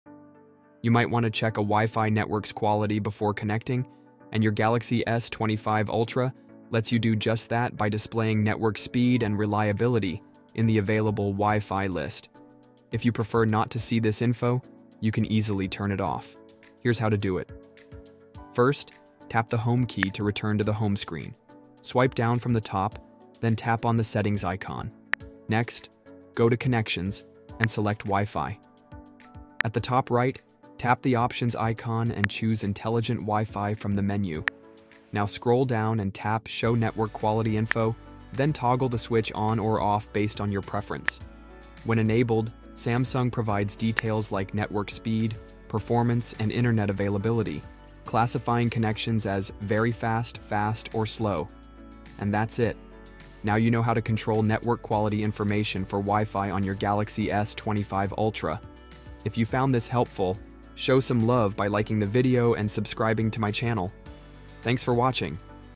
📌 How to Show or Hide Wi-Fi Network Quality Info | Samsung Galaxy S25 Ultra
Want to check a Wi-Fi network's speed and reliability before connecting? Your Samsung Galaxy S25 Ultra can display network quality details in the available Wi-Fi list. In this guide, we’ll show you how to turn this feature on or off based on your preference.

🔹 Here’s what you’ll learn:
✔️ How to access Wi-Fi settings
✔️ Steps to enable or disable network quality info
✔️ What the different Wi-Fi speed classifications mean

📌 Timestamps:
0:00 Introduction
0:17 Accessing Wi-Fi Settings
0:30 Navigating to Intelligent Wi-Fi
0:37 Enabling or Disabling Network Quality Info
0:53 Wrap-Up & Final Tips

Have any questions? Drop them in the comments below.